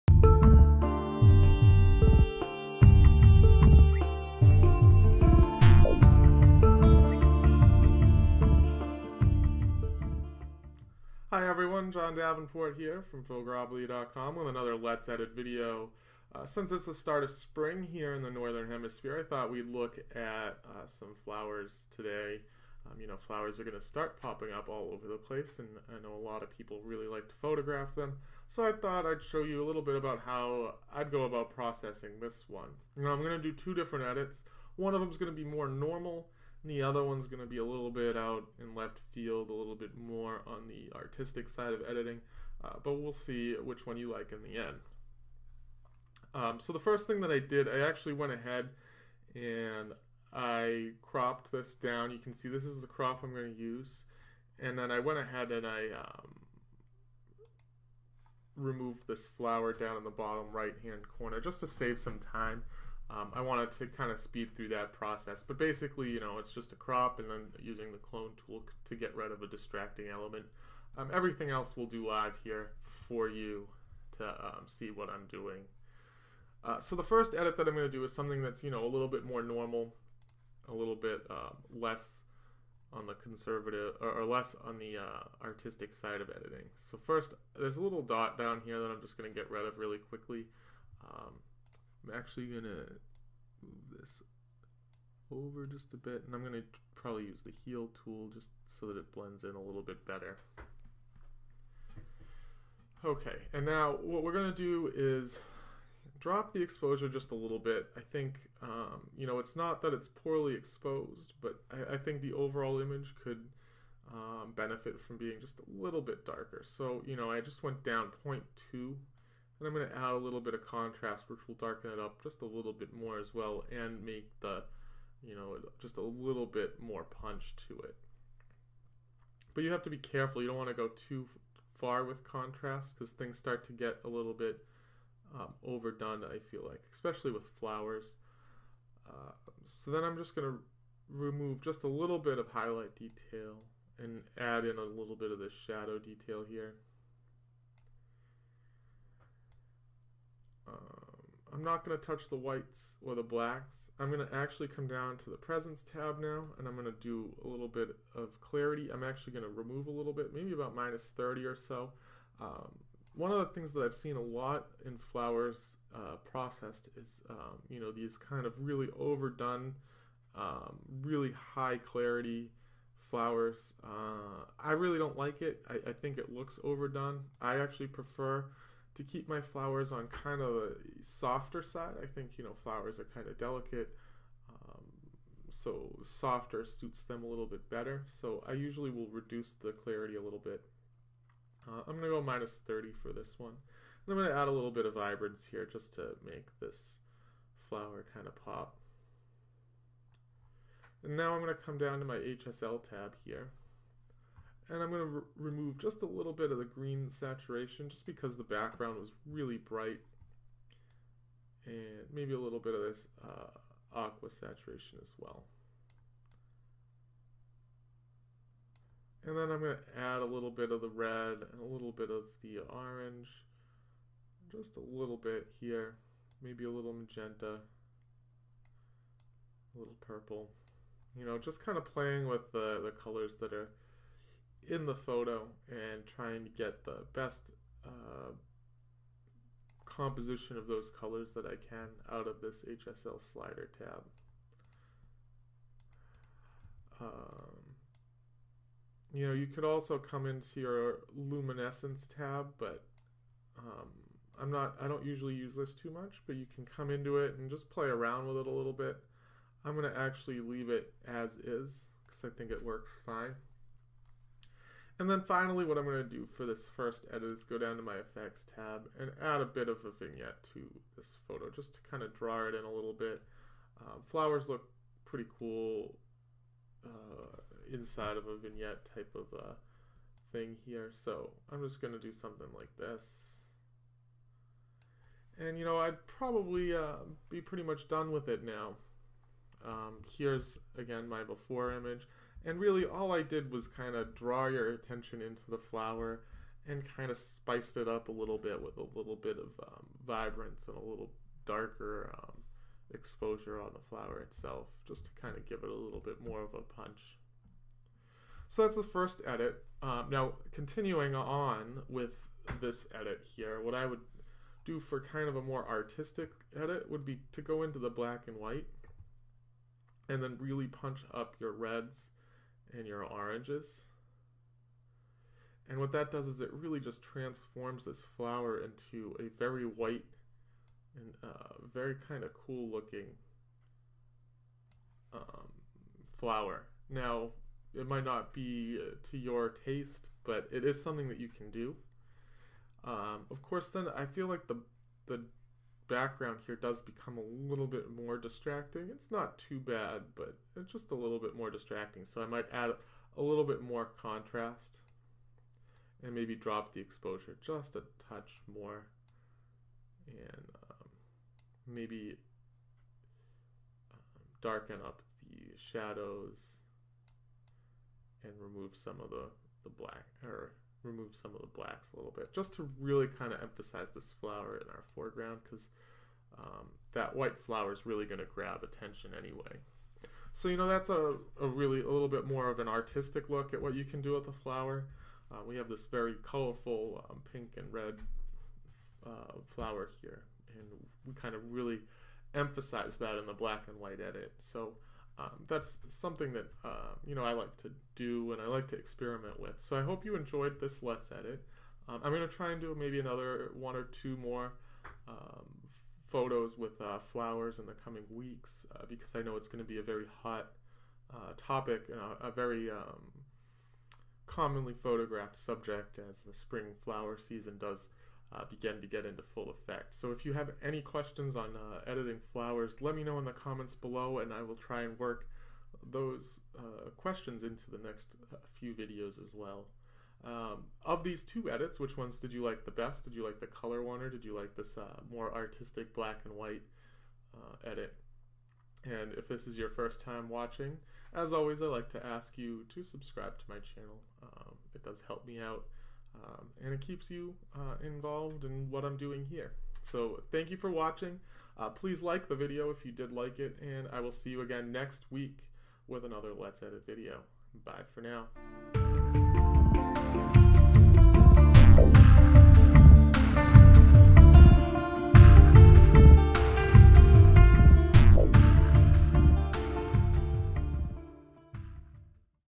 How to Edit Flower Photos in Lightroom
In the spirit of Spring (which has yet to arrive here in New England) I thought I'd show you how to edit a photograph of a flower this week on Let's Edit. Initially there really wasn't too much wrong with this photograph, and for some, it might be fine to leave it as it was, but that's not what I do and it's certainly not why I'm here today.

I create two different versions of this flower photograph one is a nice soft, colorful and vibrant version. While the other version is a more artistic take on the editing process creating a high contrast black and white image.

Which do you like best?